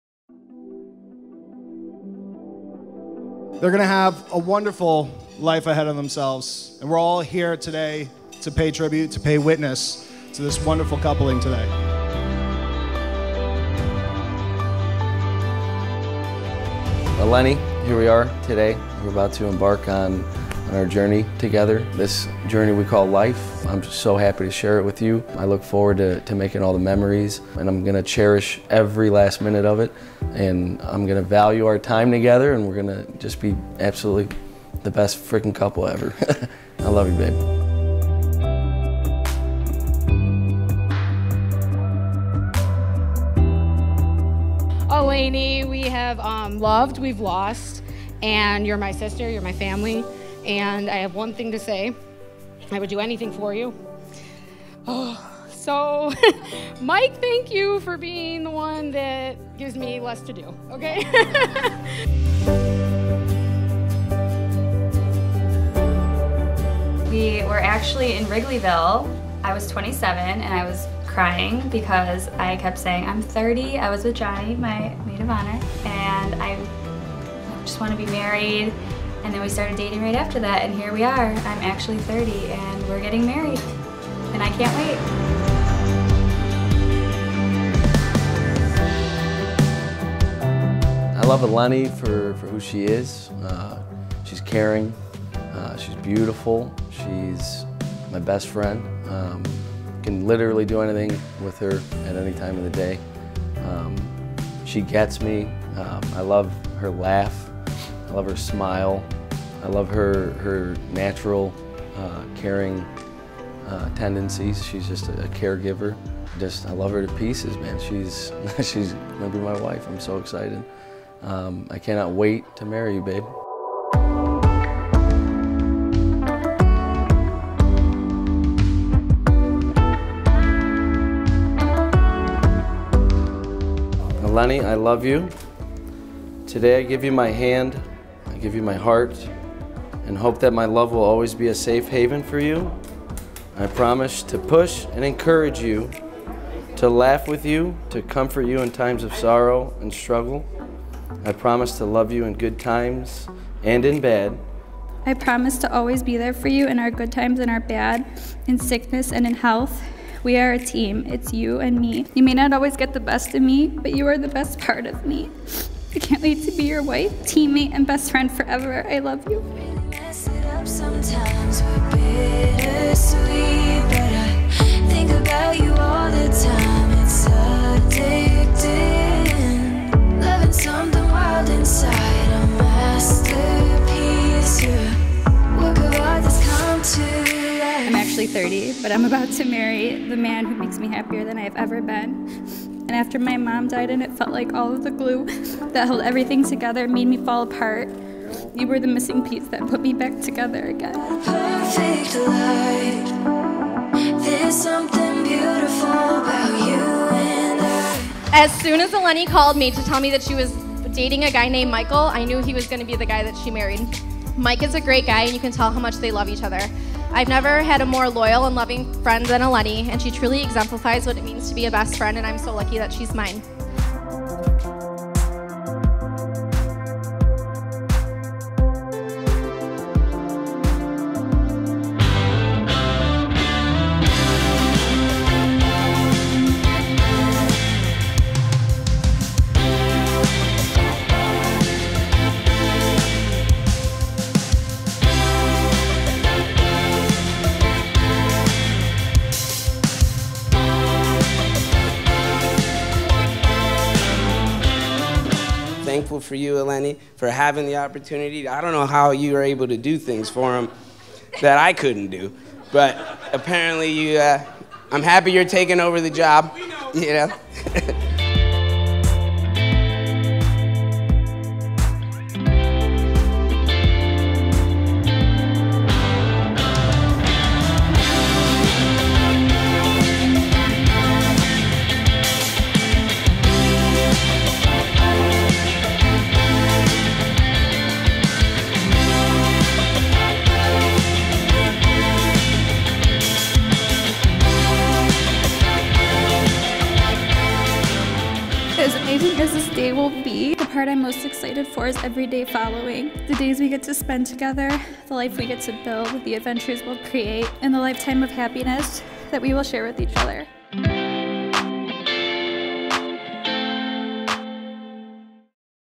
Eleni + Michael Wedding Film | Mistwood Golf Club, Romeoville, IL | White Tie Fox Weddings
Eleni + Michael's Wedding Film

Produced by White Tie Fox Weddings
Cinematography: Michael Oleshko, Marc Sanchez

Ceremony: Sts. Constantine + Helen Greek Orthodox Church
Reception: Mistwood Golf Club, Romeoville, IL
Photo: TWA SAL
Entertainment: Tone Productions | DJ Spiro
Make up: Tamara Makeup + Hair Artistry
 Hair: Lauren Hughes
Wedding Dress: Eva's Bridal
Bridesmaids Dress Designer:  David's Bridal
Tux / Groom's Suit: Men's WearHouse
Florist: Catherine's Gardens
Cake Designer:  Orland Bakery